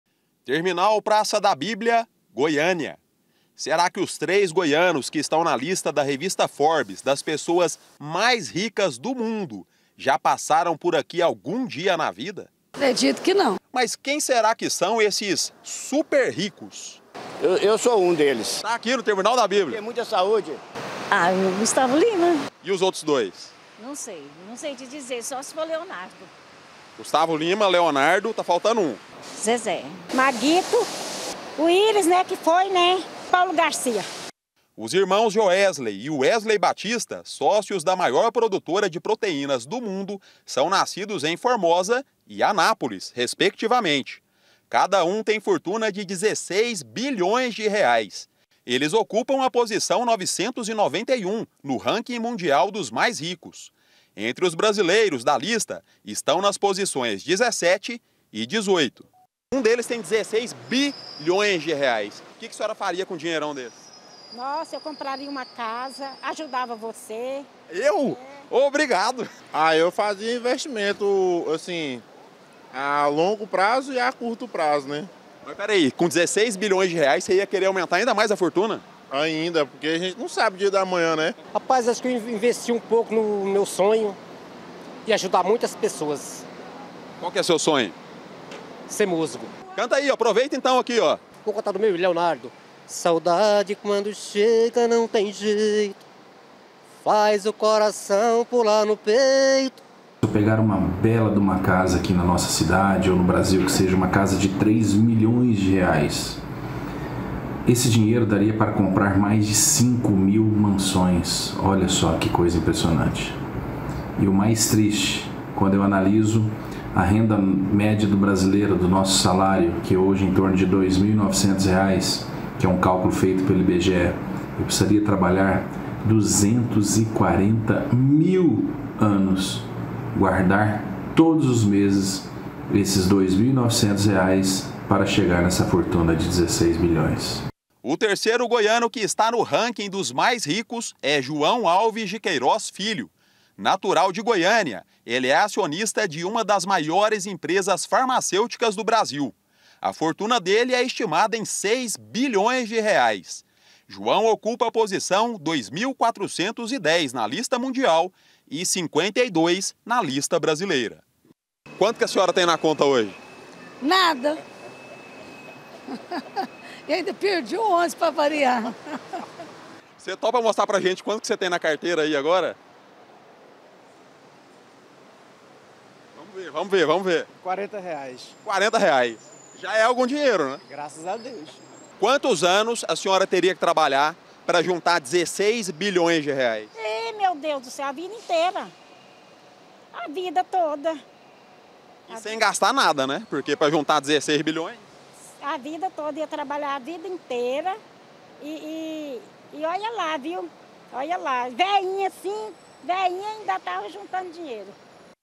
BILIONÁRIOS: TRÊS GOIANOS ESTÃO NA LISTA DOS MAIS RICOS DO MUNDO
TRÊS GOIANOS ESTÃO ENTRE OS 69 BRASILEIROS MAIS RICOS DO MUNDO, SEGUNDO O RANKING ANUAL, FEITO POR UMA REVISTA FAMOSA INTERNACIONAL. SÃO TRÊS EMPRESÁRIOS QUE, JUNTOS, TÊM UMA FORTUNA AVALIADA EM 38 BILHÕES DE REAIS.